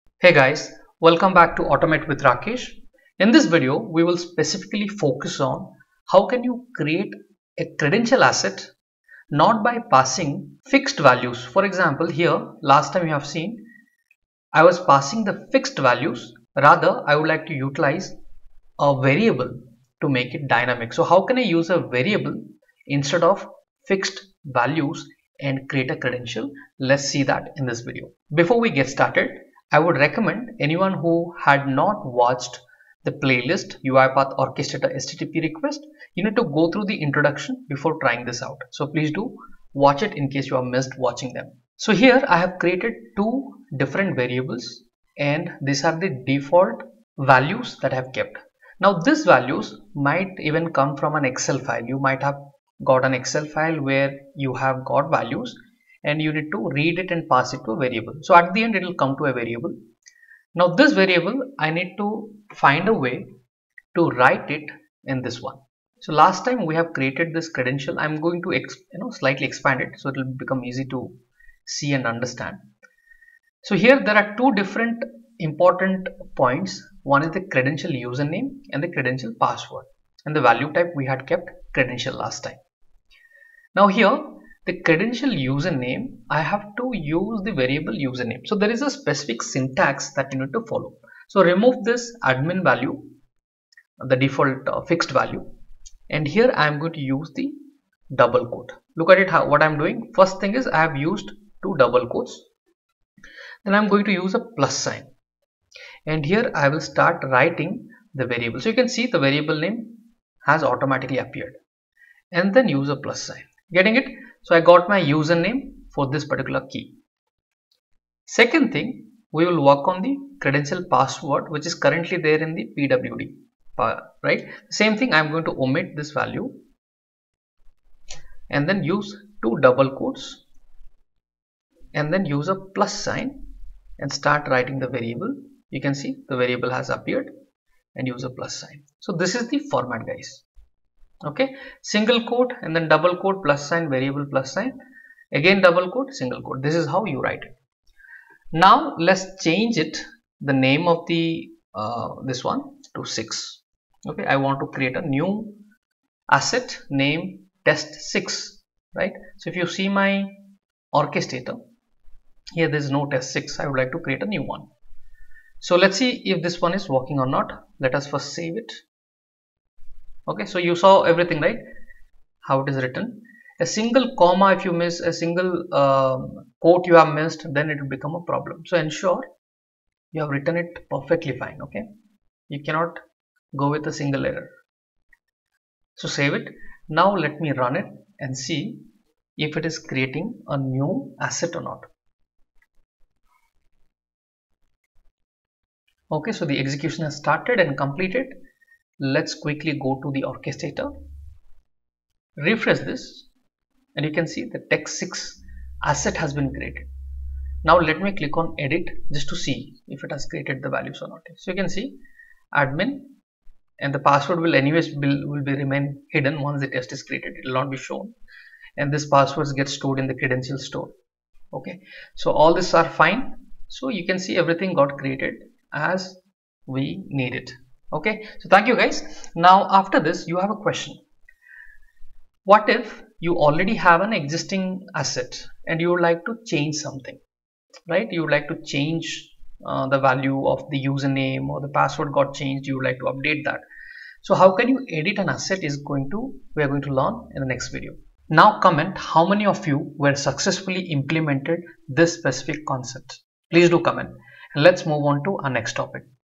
How to Use Variables in UiPath Orchestrator HTTP Request JSON Payload
You will learn how to use variables in the Json Payload of UiPath HTTP request. I will let you know the syntax for using variables in the JSON Payload and how to configure the UiPath http orchestrator  request.

Get Answers to:
How to use HTTP request in UiPath?
How do I call an orchestrator API in UiPath?